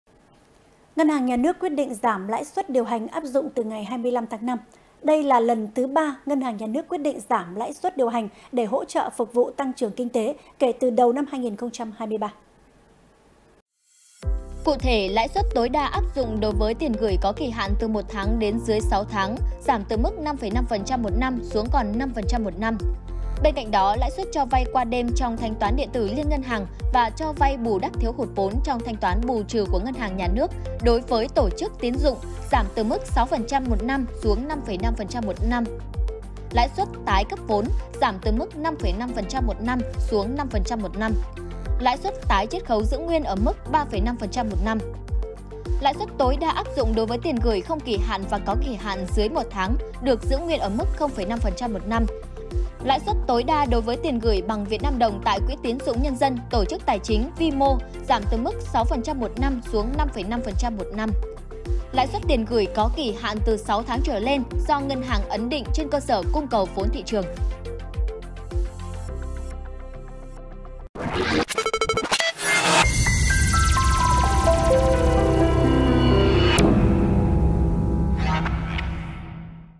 Ngân hàng Nhà nước tiếp tục giảm lãi suất điều hành lần thứ 3 từ ngày 25/5/2023 | THLC
Laocaitv.vn - Ngân hàng Nhà nước quyết định giảm lãi suất điều hành áp dụng từ ngày 25/5. Đây là lần thứ ba Ngân hàng Nhà nước quyết định giảm lãi suất điều hành để hỗ trợ phục hồi tăng trưởng kinh tế kể từ đầu năm 2023.
Đài Phát thanh - Truyền hình Lào Cai